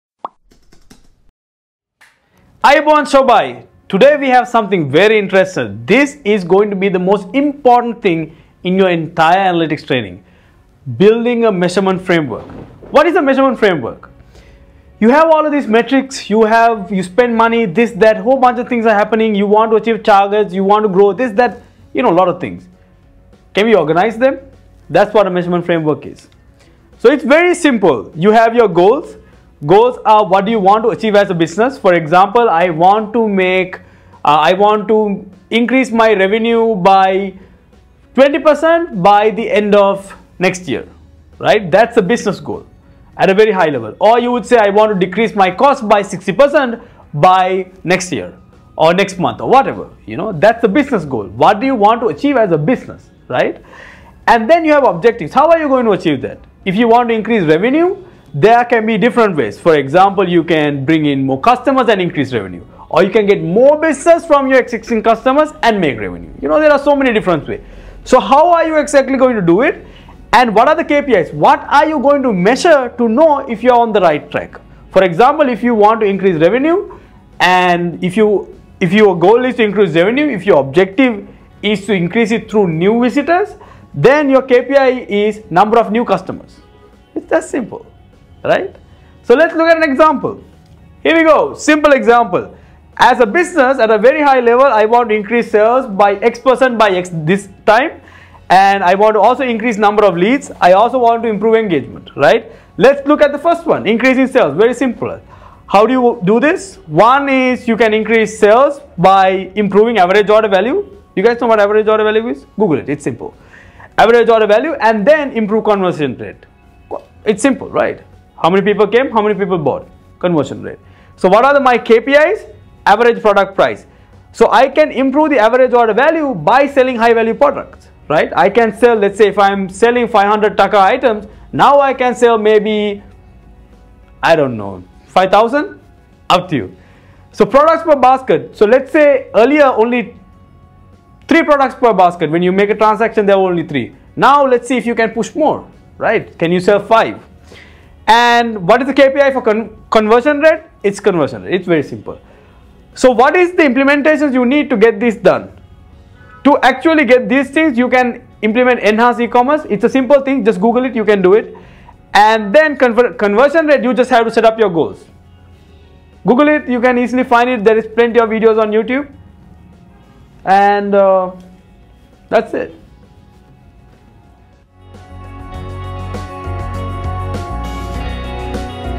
Measurement Framework | Google Analytics | Malinda Senanayake
সুপ্রিয় শিক্ষার্থীবৃন্দ, আমাদের Google Analytics সিরিজ এর এই ভিডিওতে Measurement Framework নিয়ে বিস্তারিত আলোচনা করা হয়েছে।

Instructor: Malinda Senanayake, Founder, Analytics Movement